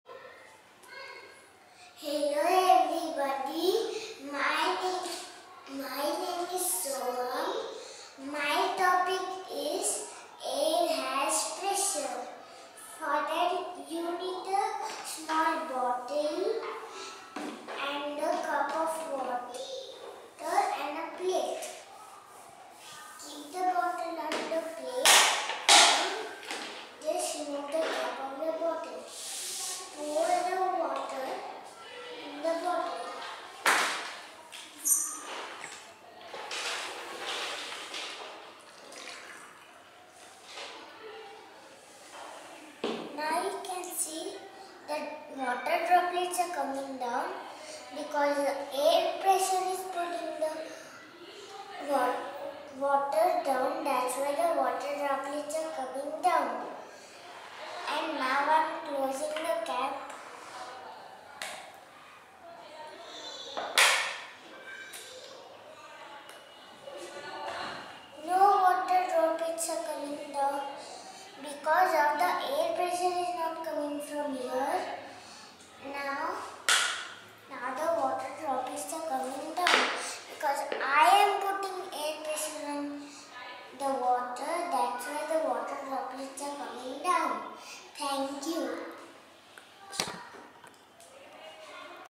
Science Carnival 2018 - Kindergarten - Air has pressure
Air pressure explained so sweetly in a magical manner. You must get your child to do this at home.